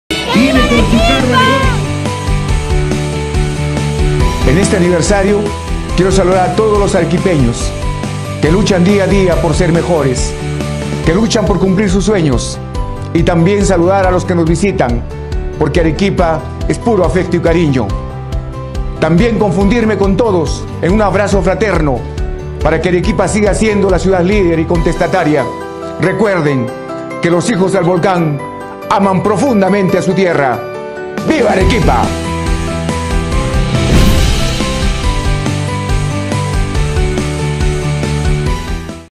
Invitación del Alcalde de Arequipa a el 476° Aniversario de la Ciudad
Te esperamos para celebrar juntos... porque en Agosto TODOS somos hermanos, nadie es extranjero.

Te invita el Dr. Alfredo Zegarra Tejada, Alcalde de Arequipa